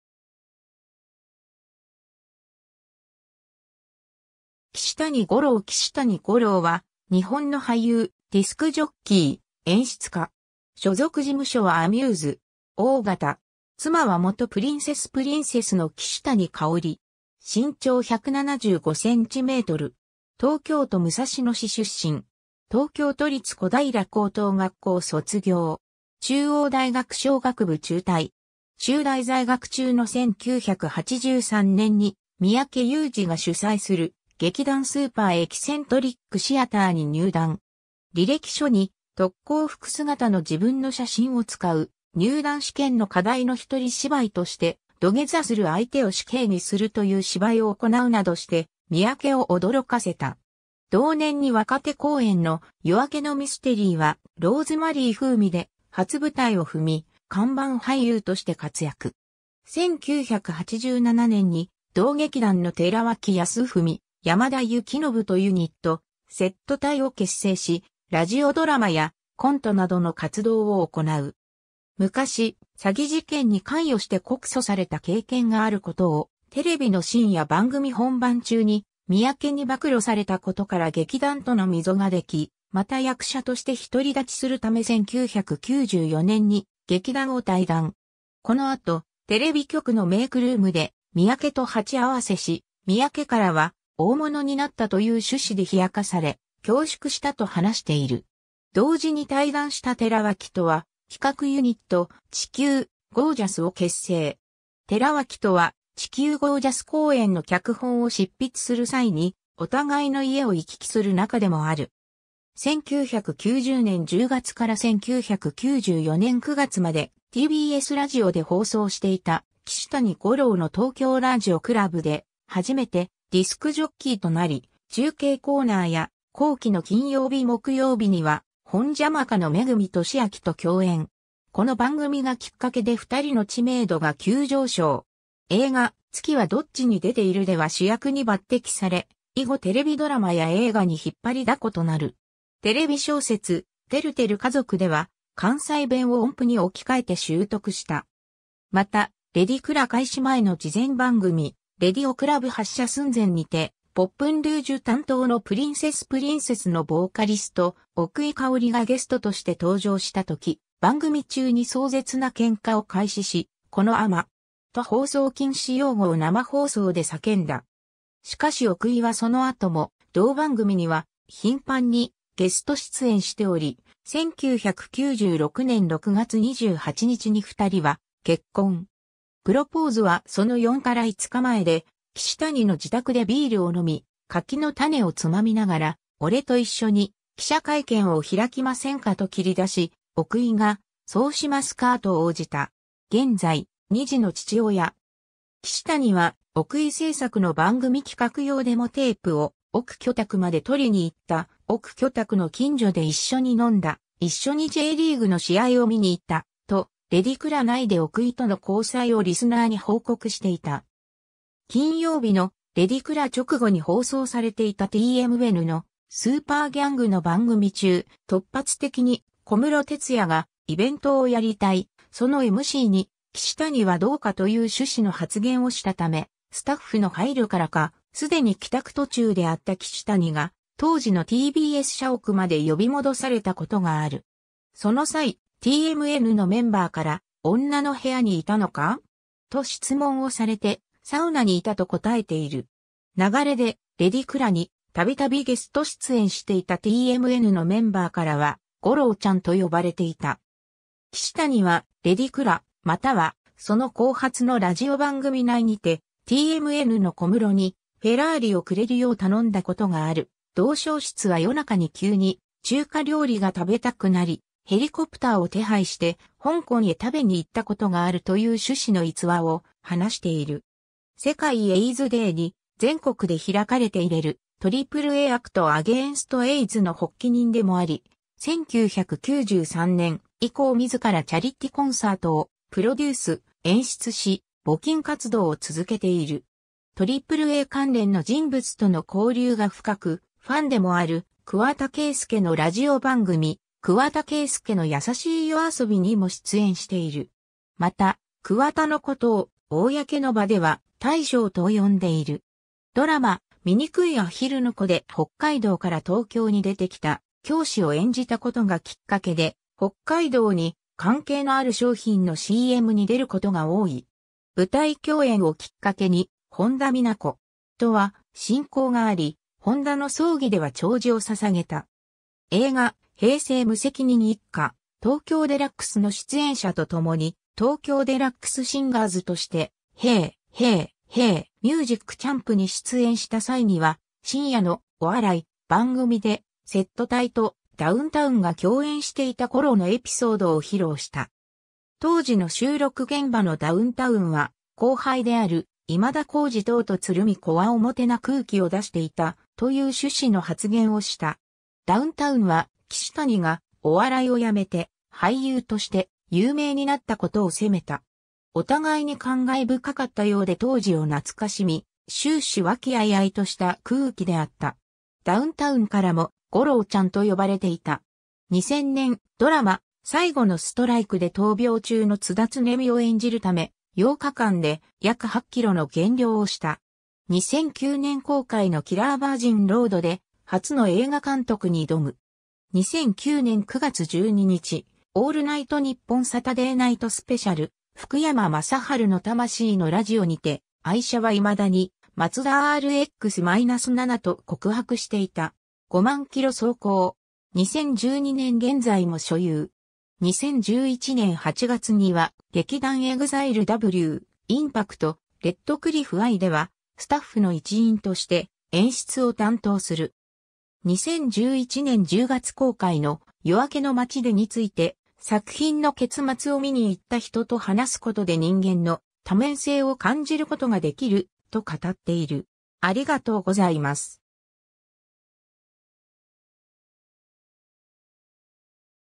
岸谷五朗
岸谷五朗 岸谷 五朗（きしたに ごろう、1964年9月27日 - ）は、日本の俳優、ディスクジョッキー、演出家。
所属事務所はアミューズ。
O型。
妻は元プリンセス プリンセスの岸谷香（旧姓・奥居）。
身長175cm。
 東京都武蔵野市出身。
東京都立小平高等学校卒業。
中央大学商学部中退。
中大在学中の1983年に三宅裕司が主宰する劇団スーパー・エキセントリック・シアターに入団。
履歴書に特攻服姿の自分の写真を使う、入団試験の課題の一人芝居として、土下座する相手を死刑にするという芝居を行うなどして三宅を驚かせた。
同年に若手公演の『夜明けのミステリーはローズマリー風味』で初舞台を踏み、看板俳優として活躍。
1987年に同劇団の寺脇康文、山田幸伸とユニット「SET隊」（せったい）を結成し、ラジオドラマやコントなどの活動を行う。
昔、詐欺事件に関与して告訴された経験がある事（のちに告訴取り下げ）をテレビの深夜番組本番中に三宅に暴露されたことから劇団との溝ができ（岸谷は何も語っていないが三宅本人が情報誌の中で語っている）、また役者として独り立ちする為1994年に劇団を退団。
この後、テレビ局のメイクルームで三宅と鉢合せし、三宅からは『大物になった』という趣旨で冷やかされ、恐縮したと話している。
同時に退団した寺脇とは、企画ユニット「地球ゴージャス」を結成。
寺脇とは、地球ゴージャス公演の脚本を執筆する際にお互いの家を行き来する仲でもある。
 1990年10月から1994年9月までTBSラジオで放送していた『岸谷五朗の東京RADIO CLUB』（以下『レディクラ』）で初めてディスクジョッキーとなり、中継コーナーや後期の金曜日→木曜日にはホンジャマカの恵俊彰と共演。
この番組がきっかけで2人の知名度が急上昇。
映画『月はどっちに出ている』では主役に抜擢され、以後（寺脇康文より若干遅く）テレビドラマや映画に引っ張りだことなる。
テレビ小説「てるてる家族」では関西弁を音符に置き換えて習得した。
 また、『レディクラ』開始前の事前番組（レギュラー開始直前の週に番組紹介を目的とした帯番組が放送された）「レディオクラブ発射寸前」にて『ポップンルージュ』担当のプリンセス プリンセスのヴォーカリスト奥居香がゲストとして登場した時、番組中に壮絶な喧嘩を開始し「このアマ!」と放送禁止用語を生放送で叫んだ。
しかし奥居はその後も同番組には頻繁にゲスト出演しており、1996年6月28日に2人は結婚。
プロポーズはその4 - 5日前で、岸谷の自宅でビールを飲み、柿の種をつまみながら「オレと一緒に記者会見を開きませんか」と切り出し、奥居が「そうしますかー」と応じた。
現在、2児の父親。
岸谷は、奥居制作の番組企画用デモテープを奥居宅まで取りに行った、奥居宅の近所で一緒に飲んだ、一緒にJリーグの試合を観に行った、と『レディクラ』内で奥居との交際をリスナーに報告していた。
 金曜日の『レディクラ』直後に放送されていたTMNの『スーパーギャング』の番組中、突発的に小室哲哉が、『（小規模の）イベントをやりたい』、『そのMCに岸谷はどうか』という趣旨の発言をしたため、スタッフの配慮からか既に帰宅途中であった岸谷が当時のTBS社屋（TMNが『スーパーギャング』を生放送していたラジオスタジオ）まで呼び戻されたことがある。
その際、TMNのメンバーから『女の部屋にいたのか？』と質問をされて、『サウナにいた』と答えている。
流れで『レディクラ』に度々ゲスト出演していたTMNのメンバーからは、『五朗ちゃん』と呼ばれていた。
 岸谷は『レディクラ』、又は、その後発のラジオ番組内にて、『TMNの小室にフェラーリをくれるよう頼んだことがある』、『同小室は夜中に急に中華料理が食べたくなり、ヘリコプターを手配して香港へ食べに行ったことがある』という趣旨の逸話を話している。
 世界エイズデーに全国で開かれていれる『AAA〜アクト・アゲインスト・エイズ』の発起人でもあり、1993年以降（近年は主に武道館で）自らチャリティコンサートをプロデュース・演出し、募金活動を続けている。
AAA関連の人物との交流が深く、ファンでもある桑田佳祐のラジオ番組『桑田佳祐のやさしい夜遊び』にも出演している。
また、桑田の事を公の場では「大将」と呼んでいる。
 ドラマ『みにくいアヒルの子』で北海道から東京に出てきた教師...